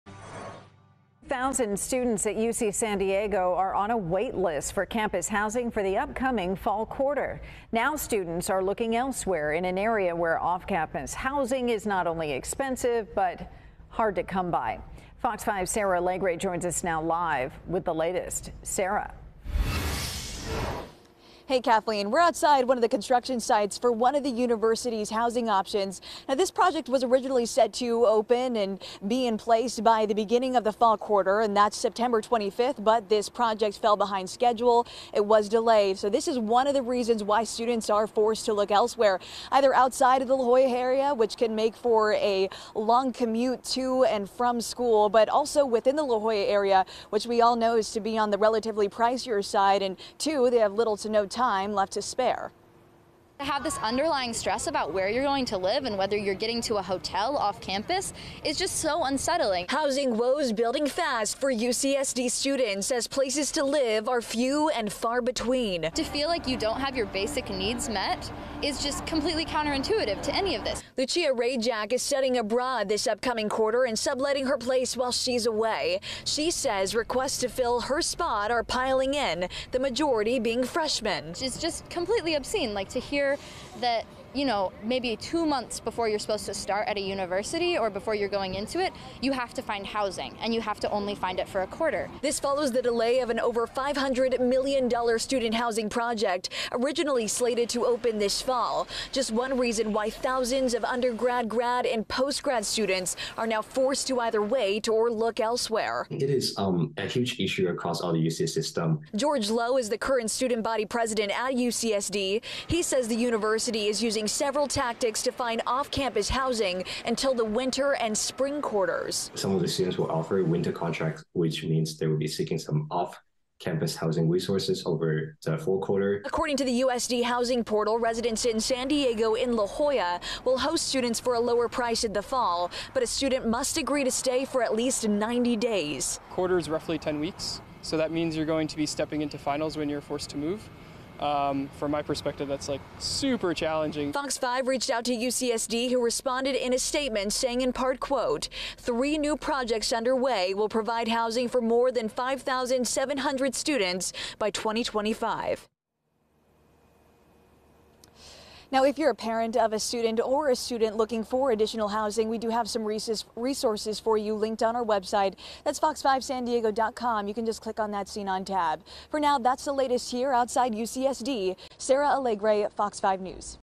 UCSD Housing Shortage
Sarah Alegre reports on the waitlist for campus housing.